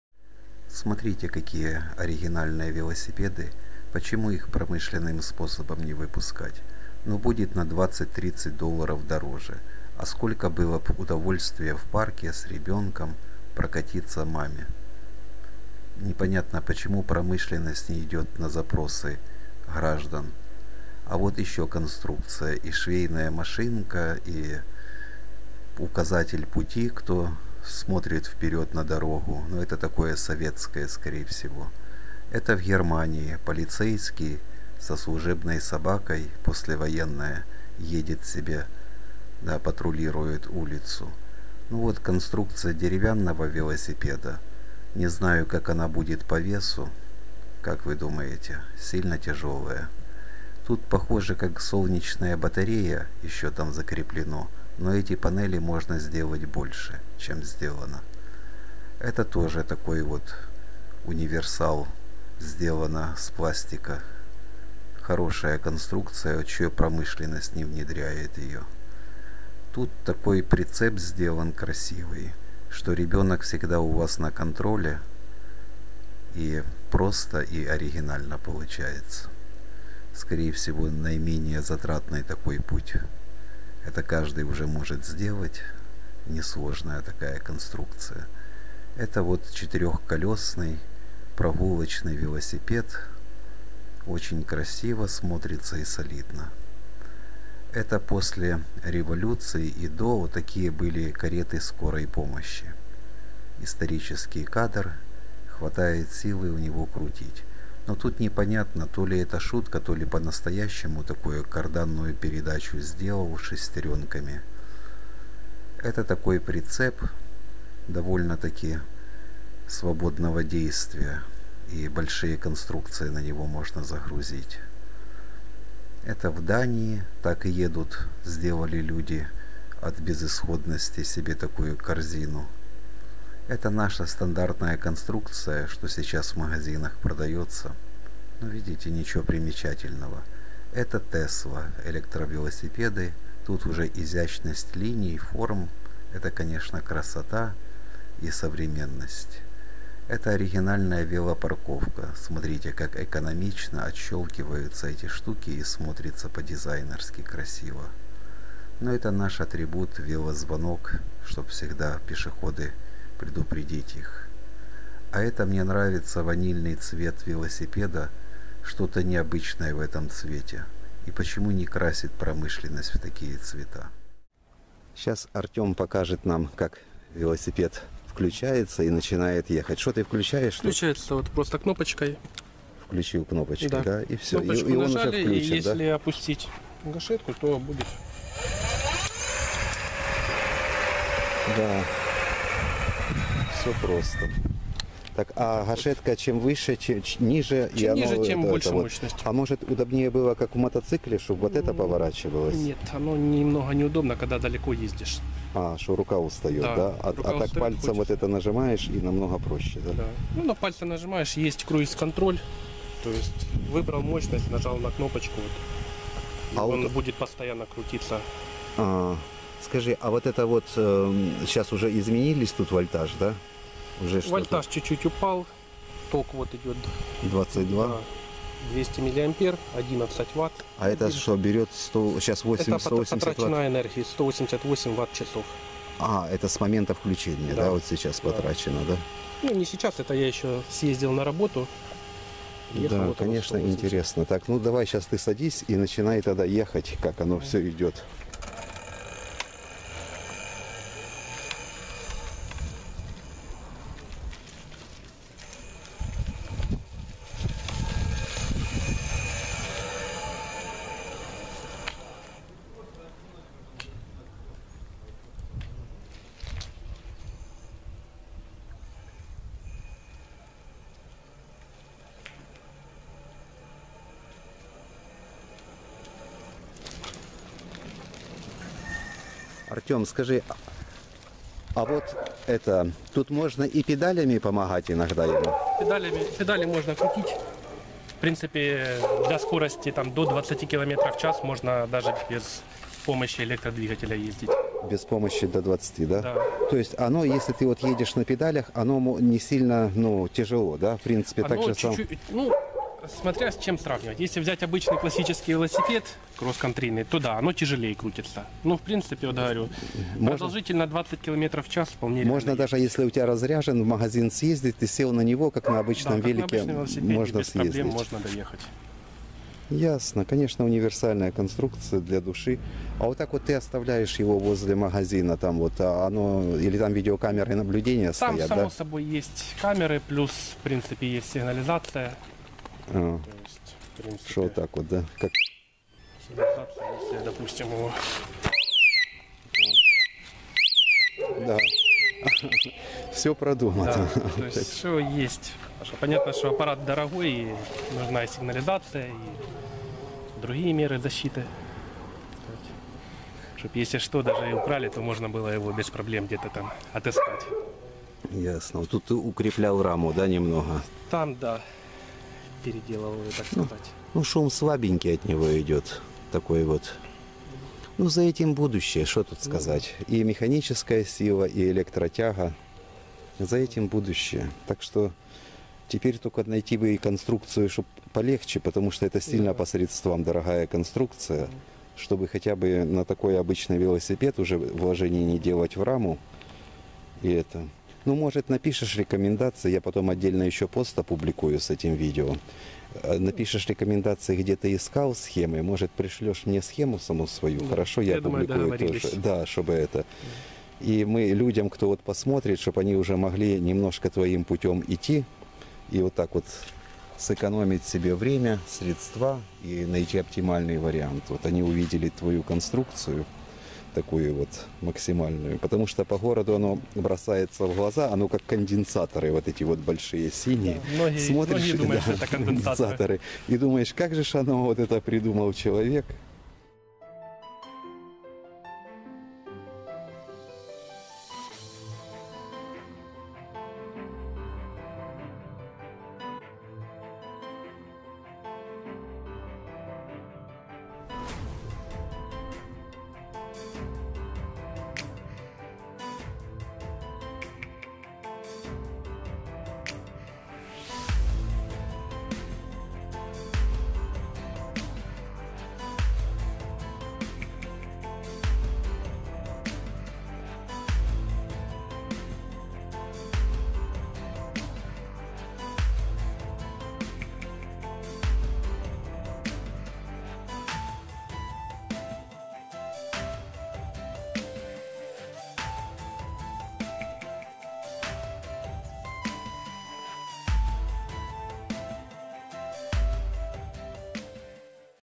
Велоконструкции оригинальные от прошлого до наших дней: дерево, металл, пластик, электро фото видео
Всё красиво и оригинально. Только на улицах их мало ))
Почему?